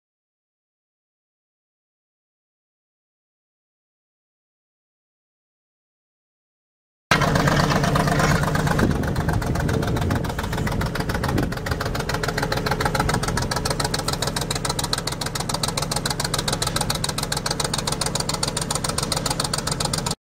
1969 International Loadstar 1700 drilling rig truck for sale | sold at auction June 25, 2015
1969 International Loadstar 1700 drilling rig truck
12,372 miles on odometer
International eight cylinder gas engine
Heat
Manual windows and locks
Vinyl interior
Sterling BT7 earth boring machine
Ford six cylinder gas engine
Swinging turret
12" and 18" augers
Hydraulic drive
Single fuel tank
Mud flaps
Spring suspension
Hydraulic brakes
151" wheelbase
10R20 tires
GVWR 21,500 lbs
486070H919964

It was located in New Ulm, MN, and we sold it to the highest bidder for $1,155.